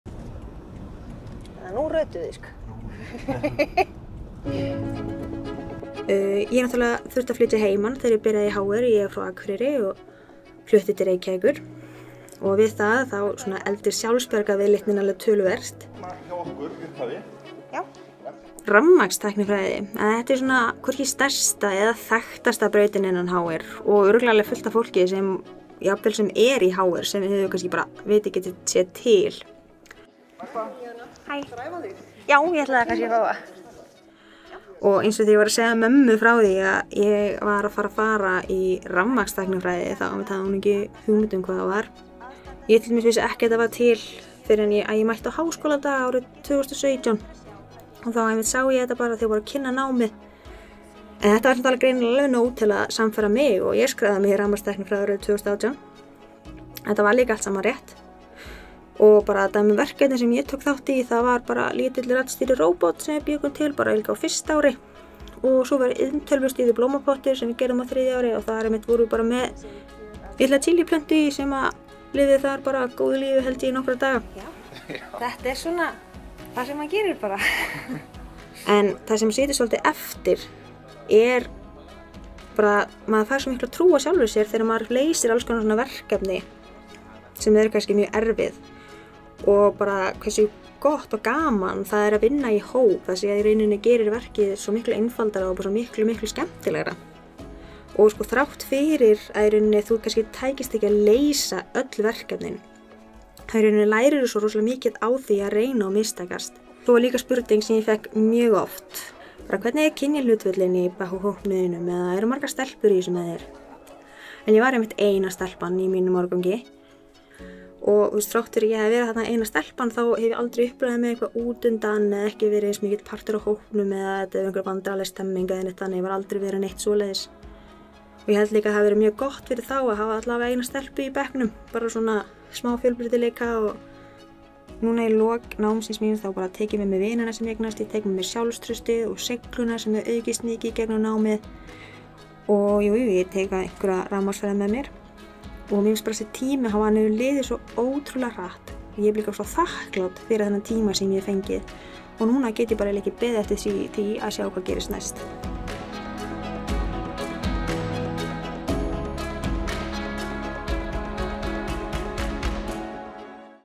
Júlíana Ingimundardóttir „Get ekki beðið eftir að sjá hvað gerist næst” | Háskólinn í Reykjavík
Júlíana Ingimundardóttir kom beinustu leið eftir stúdentspróf á Akureyri í krefjandi nám í rafmagnstæknifræði í HR. Nú er náminu lokið, Júlíana er reynslunni og gráðunni ríkari og allir vegir færir.